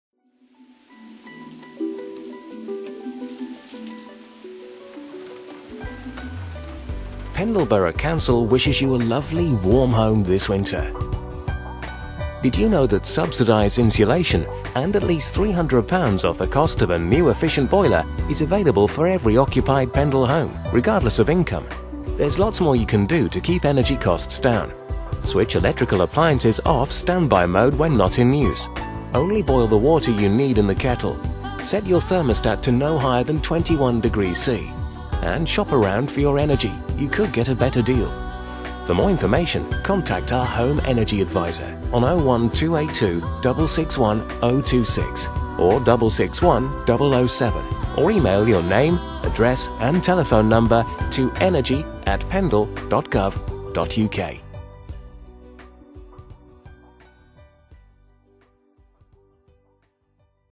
Keeping warm in winter
Advice, guidance and support to help you keep warm this winter from Pendle Borough Council.